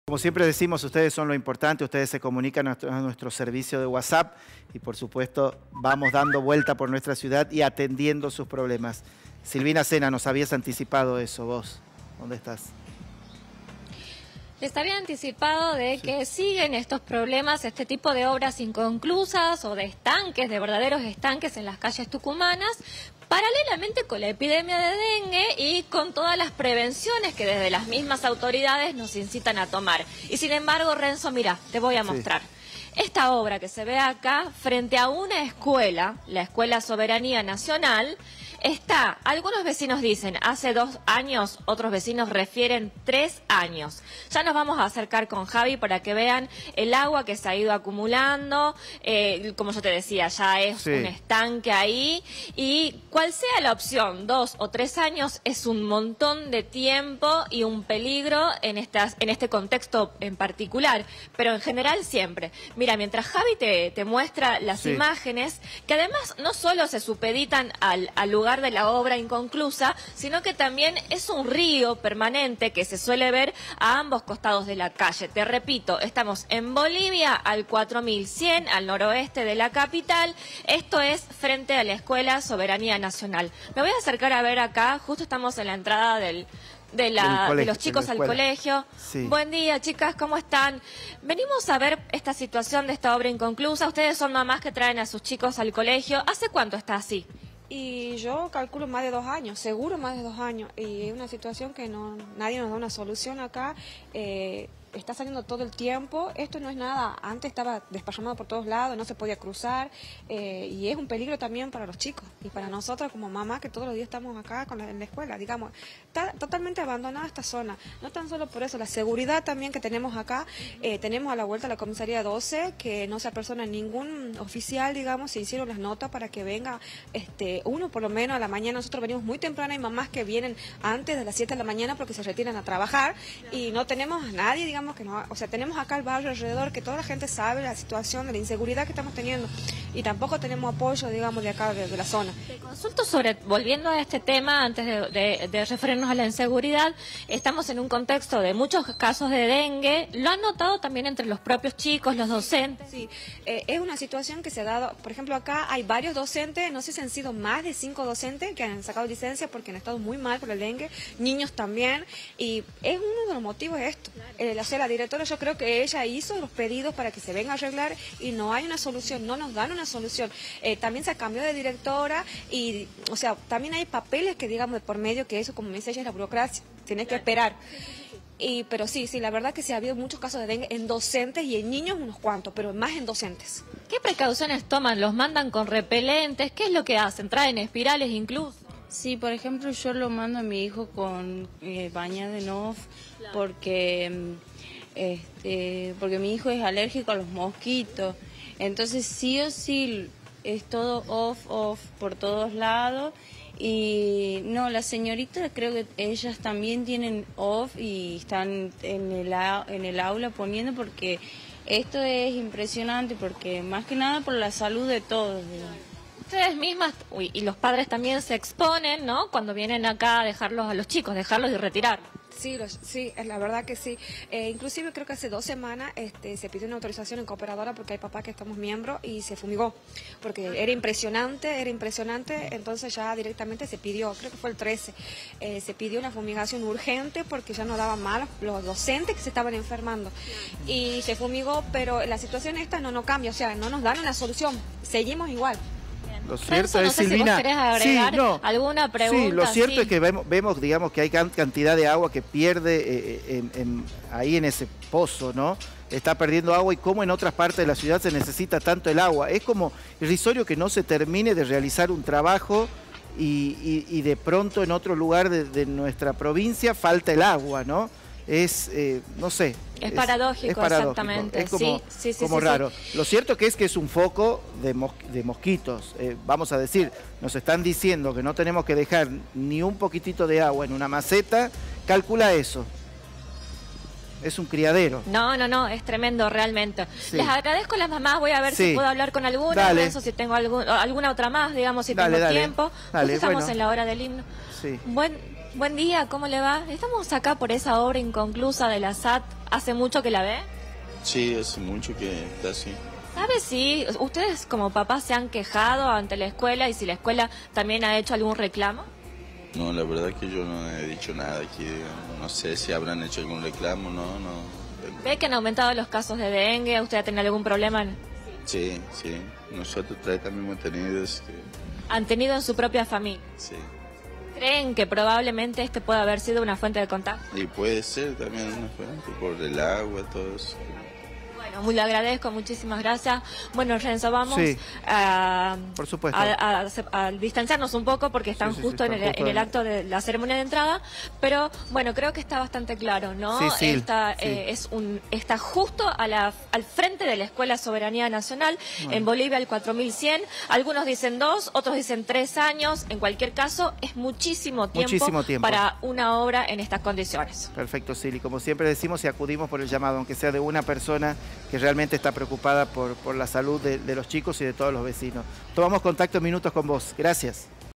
OBRA INCONCLUSA DE LA SAT
Vecinos de la calle Bolivia al 4.100 reclaman y piden a las autoridades una solución urgente para una obra inconclusa que dejó la Sociedad Aguas del Tucumán (SAT).

A este pedido se suman también los padres y las madres de los alumnos de la Escuela Soberanía Nacional, quienes tienen al frente del establecimiento este pozo que pierde agua constantemente. Indicaron que esta obra se encuentra inconclusa hace casi tres años y se convirtió en un criadero de mosquitos.